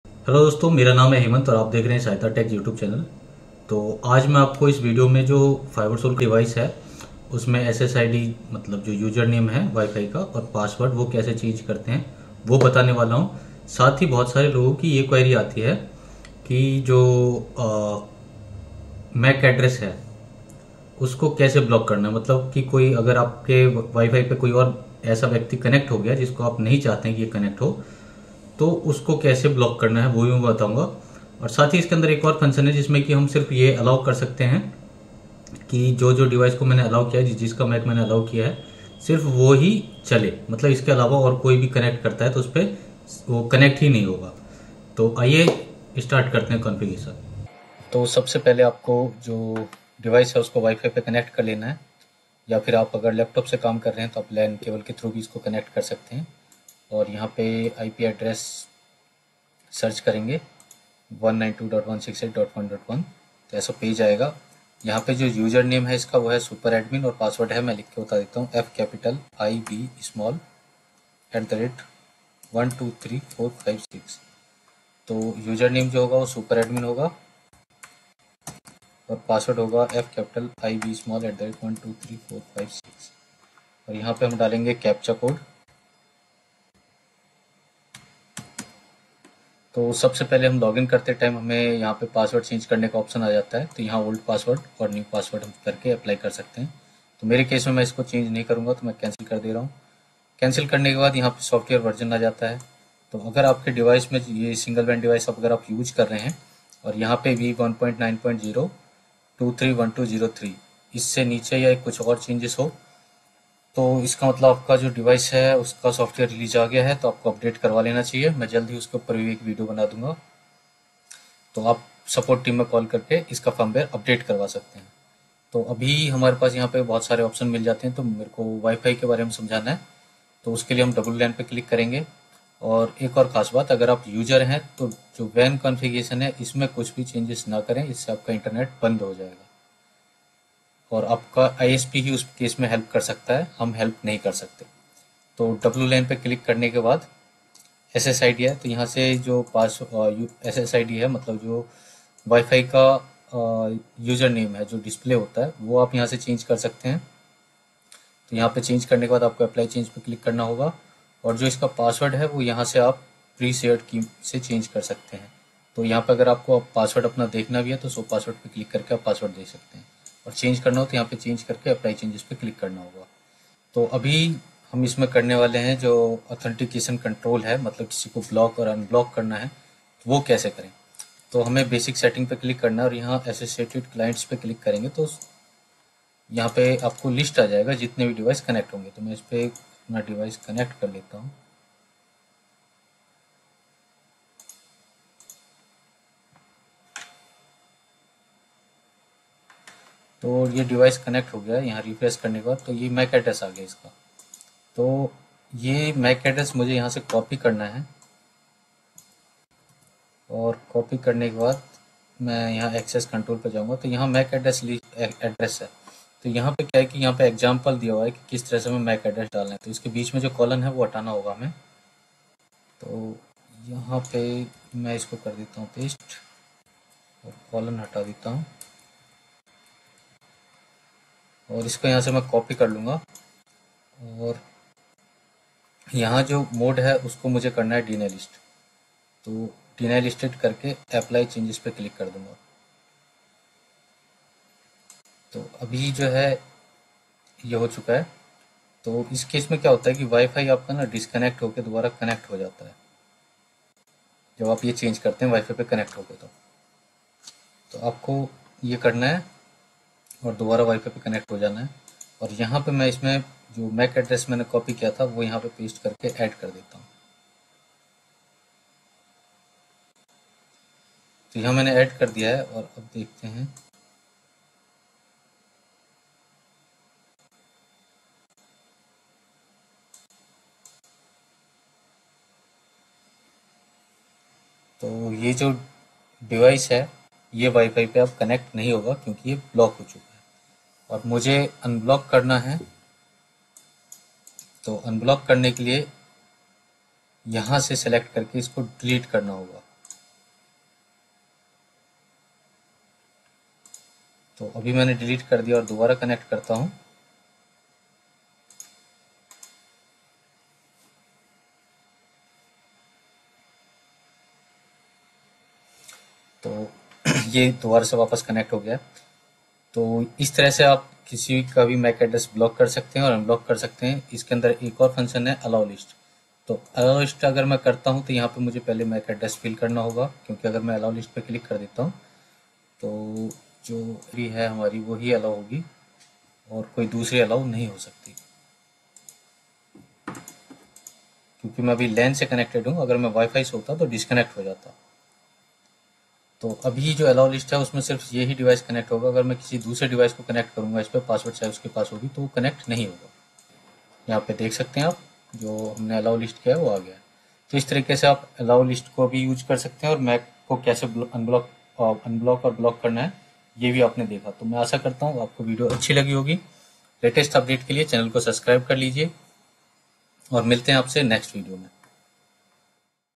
how to block user on wifi | राऊटर पर अनजान लोगों को कैसे block करे ? model FS-HGR1100W
For basic configuration tap on given links.